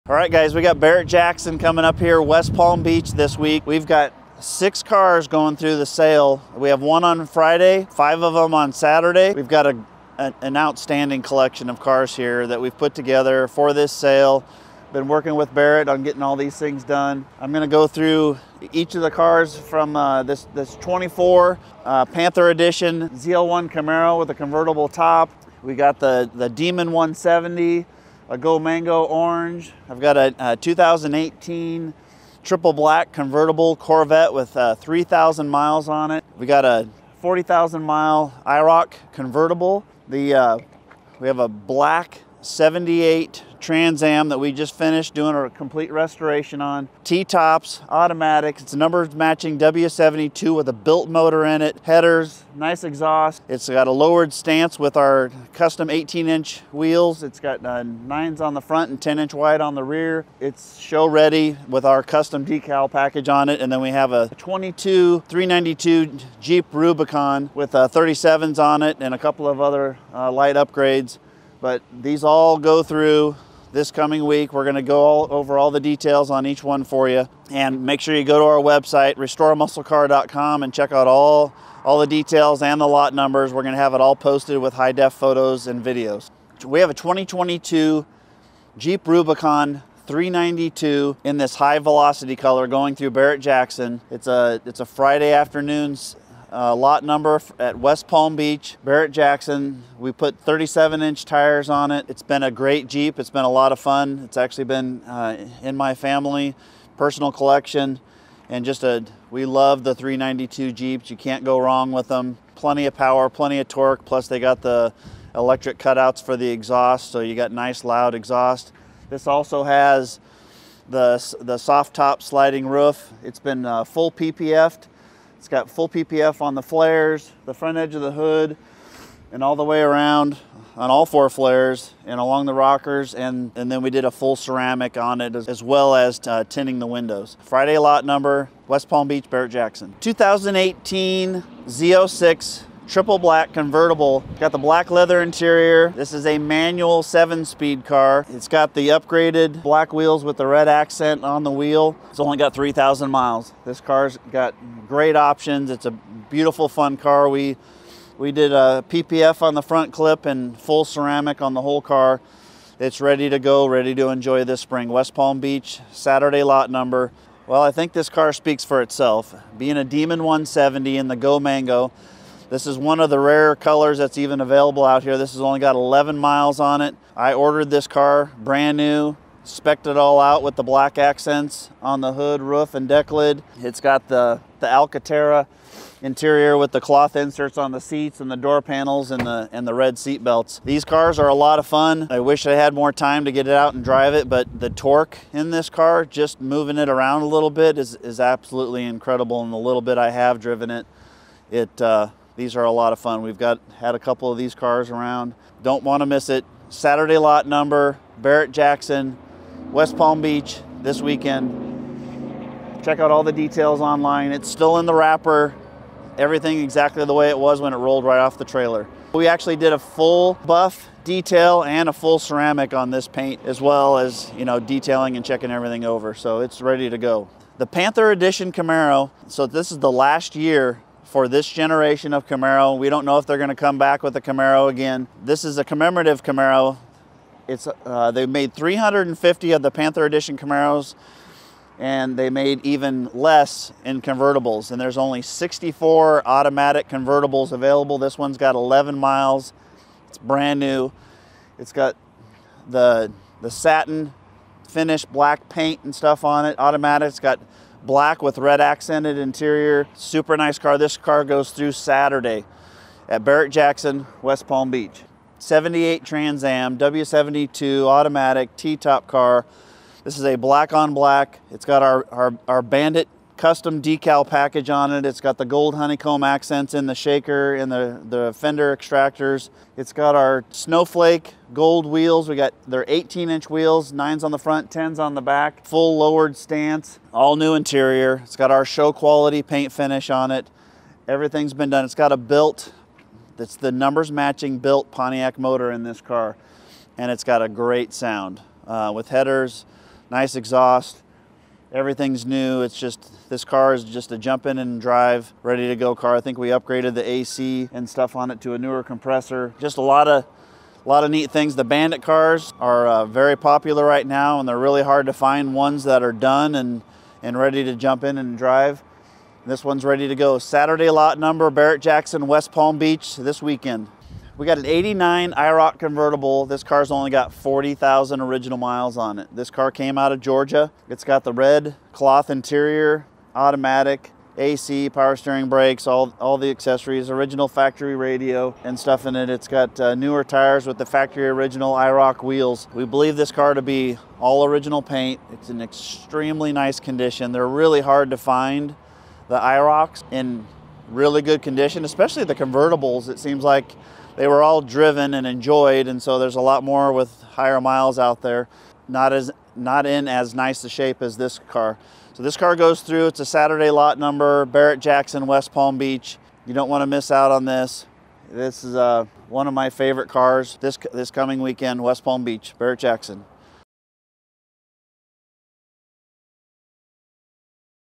2024 Barrett Jackson Palm Beach Restore A Muscle Car [LINE UP]
Lot #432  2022 JEEP WRANGLER UNLIMITED RUBICON

Lot #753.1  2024 CHEVROLET CAMARO ZL1 COLLECTOR'S EDITION CONVERTIBLE